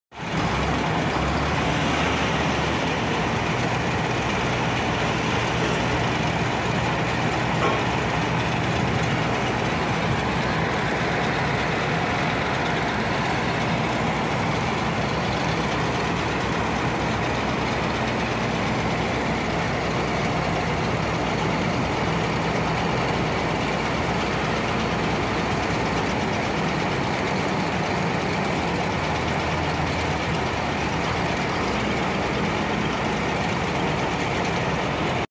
Bulk Cargo Discharging by Dump truck
Bulk Cargo loading In Warehouse Operation in Port Sudan